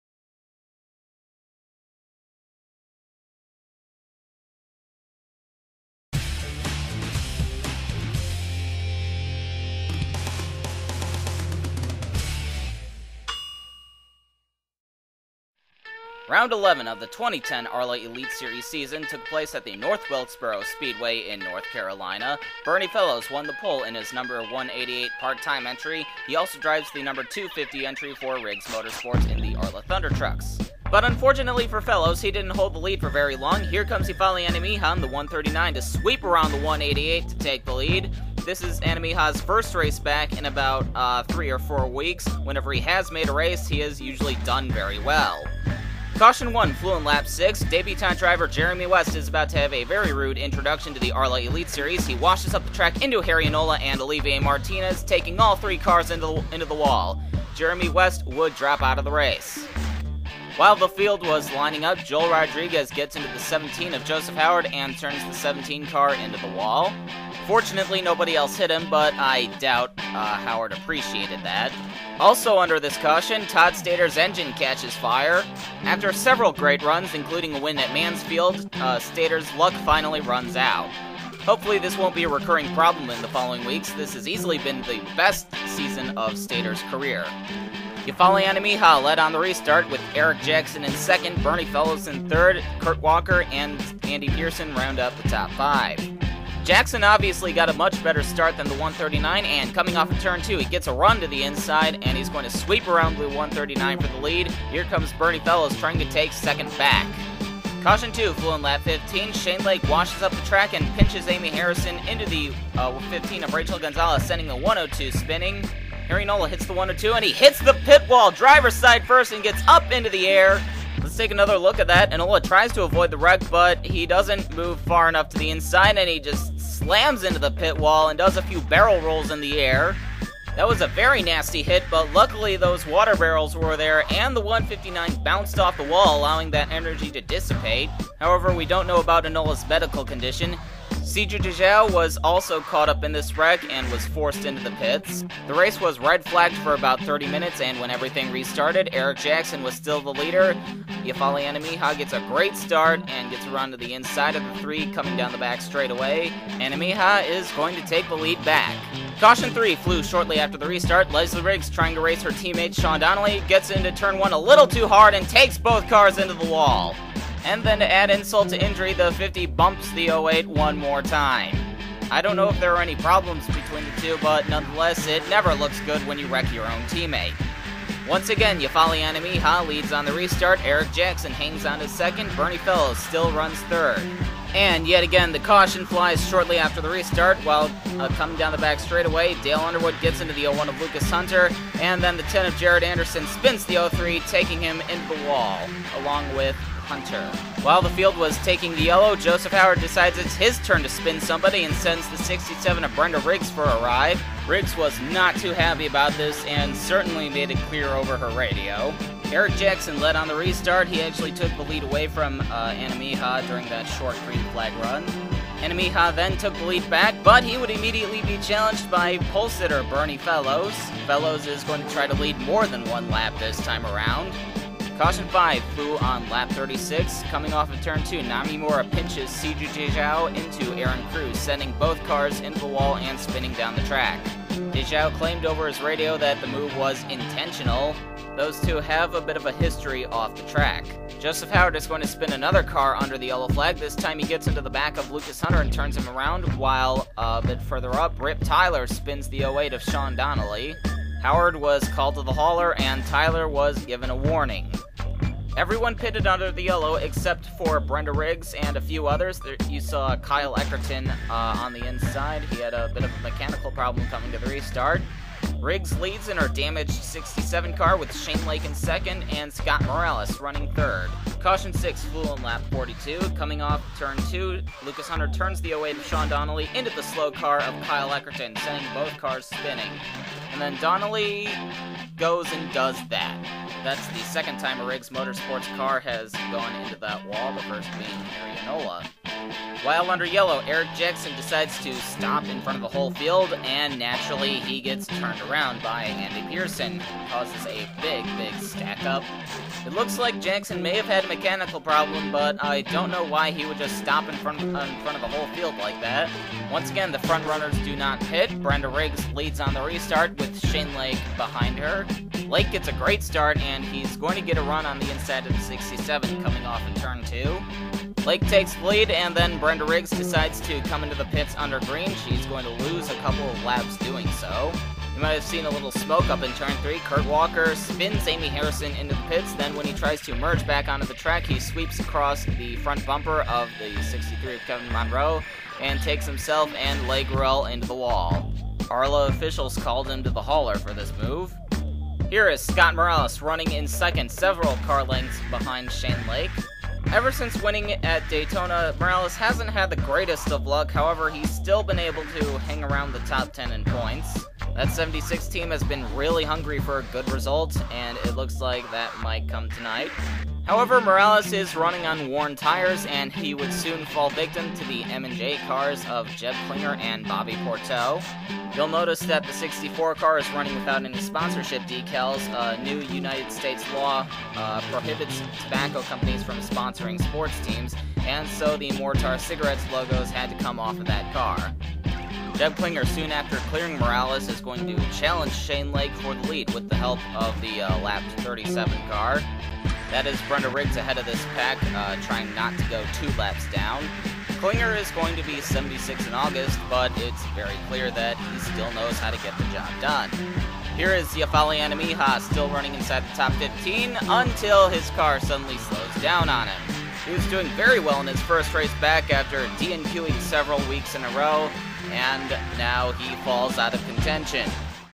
2010 ARLA Elite Series: Race 11.1 (North Wilkesboro 150)
Part 1 of the Elite Series race at the North Wilkesboro Speedway.